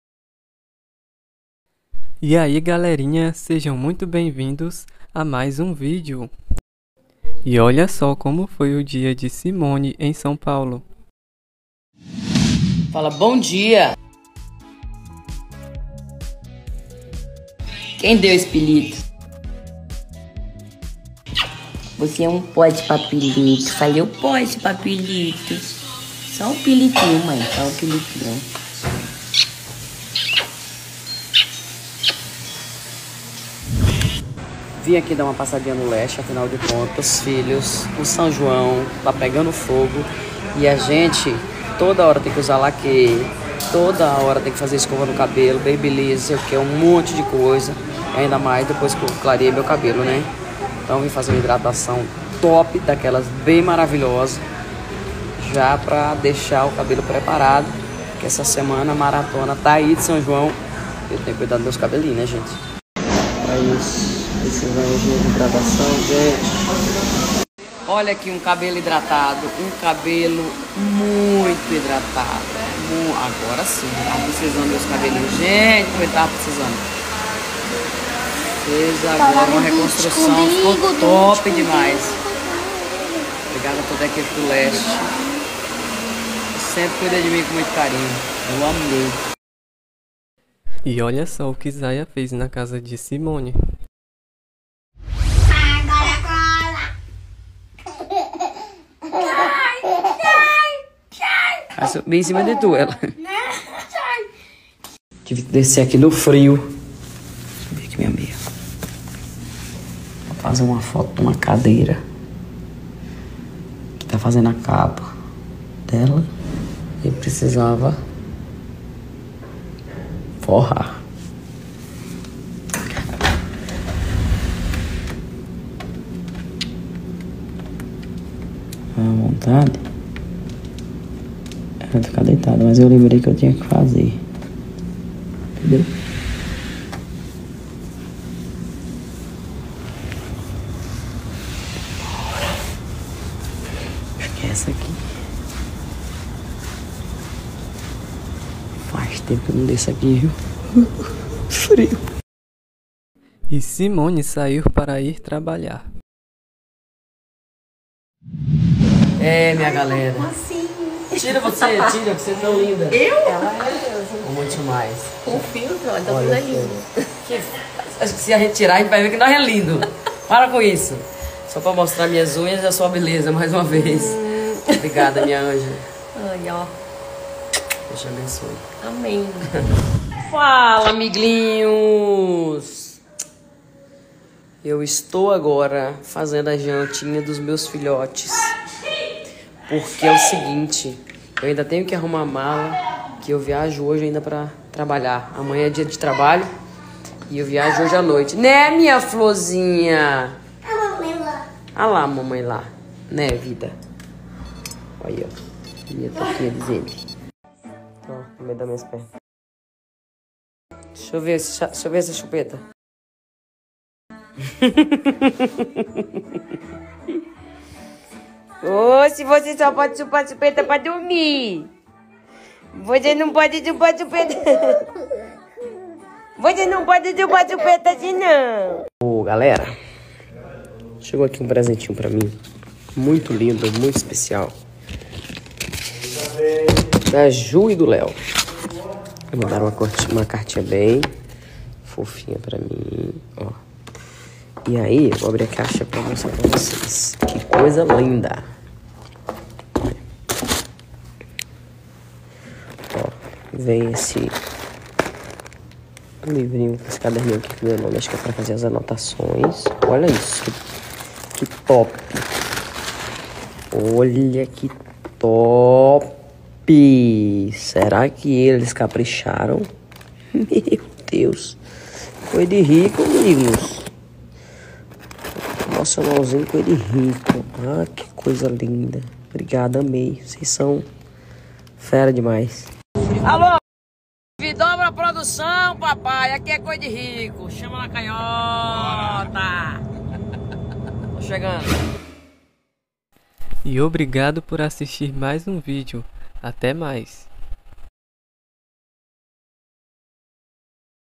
OLHA SÓ O APEGO DE ZAYA À SIMONE 😍
Simone Mendes foi arrumar seu cabelo e suas unhas. No outro dia Simone mostrou o quanto Zaya é apegada a ela. Simone viajou a trabalho com seu empresário.

Os comentários nos vídeos são de autoria própria deste canal e são adicionados com o objetivo de agregar valor aos vídeos.